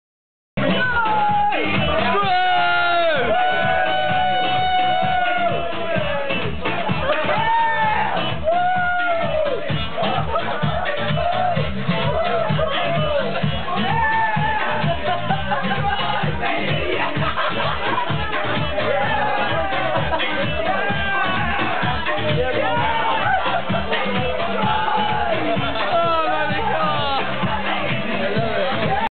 Another Stroh Victim!
Whilst travelling my our new friend takes stroh for the first time AHOOOO PARTY ON!!!